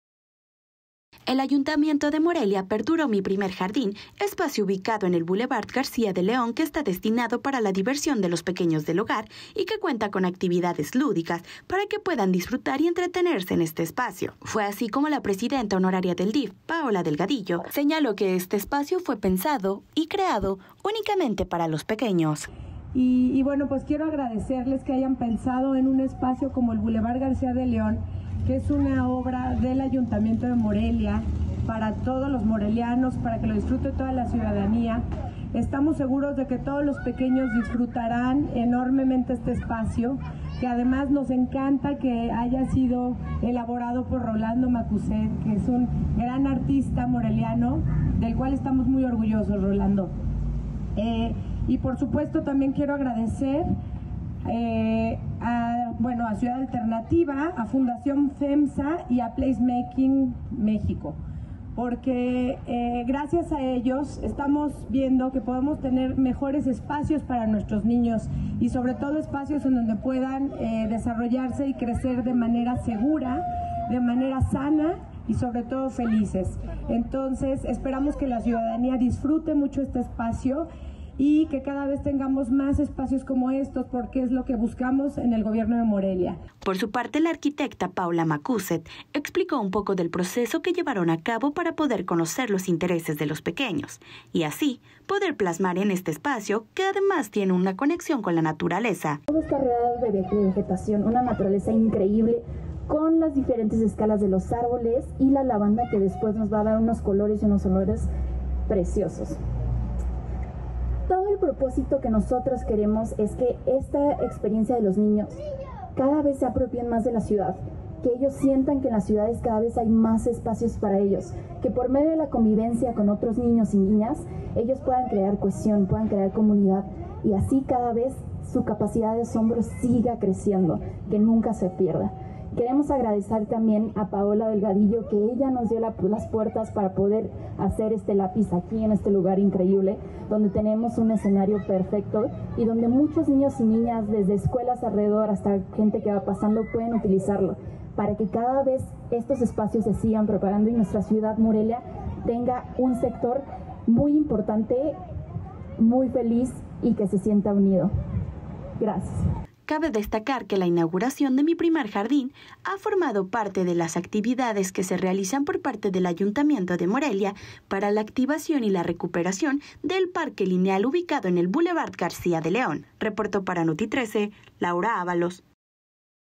"Mi Primer Jardín" En El Boulevard De Morelia
Morelia 🔴 Aperturan "Mi primer jardín" en el Boulevard de Morelia, espacio dedicado para que los niños morelianos puedan tener un lugar seguro de esparcimiento con herramientas lúdicas en torno a la naturaleza.

Laura Ávalos | Notitrece